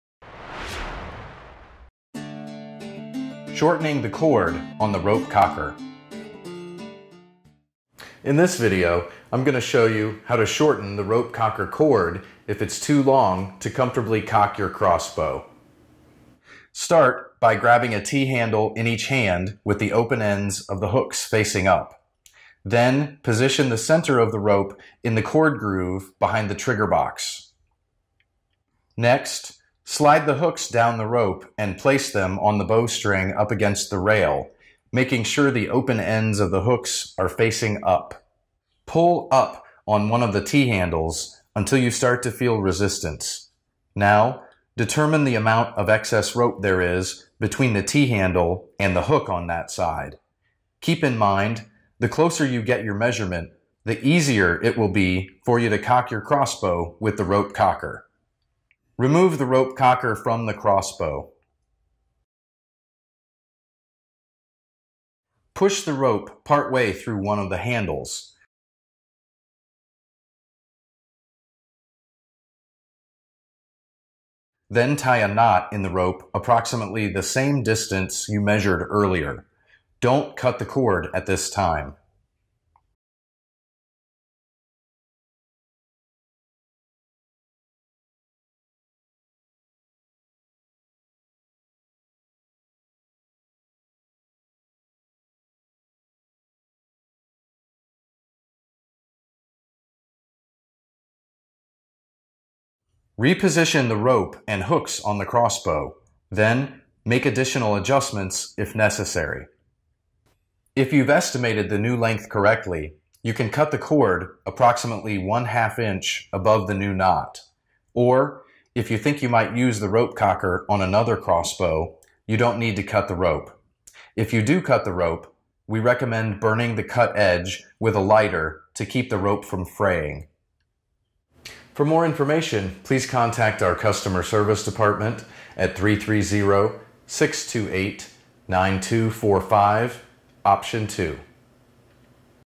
How to Shorten the Cord on a TenPoint Rope-Cocker | TenPoint Crossbows
How to Shorten the Cord on a TenPoint Rope-Cocker.